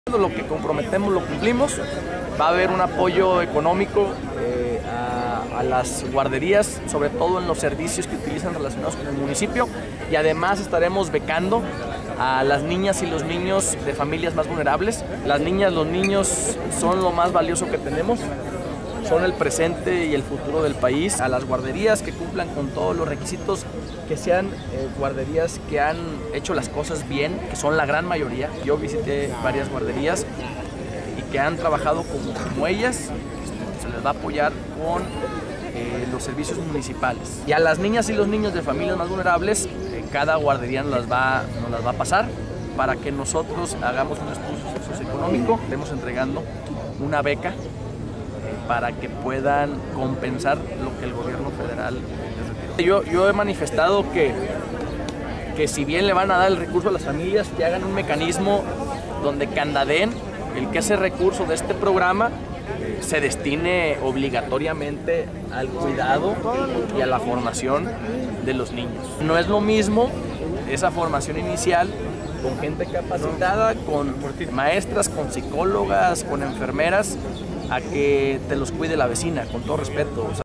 BECARÁ MUNICIPIO A NIÑOS EN GUARDERÍAS
El alcalde de Saltillo, Manolo Jiménez Salinas, anunció la puesta en marcha de un programa para apoyar a las guarderías locales y becar a niños de familias en situación vulnerable, para -dijo- "compensar lo que el Gobierno Federal les quitó".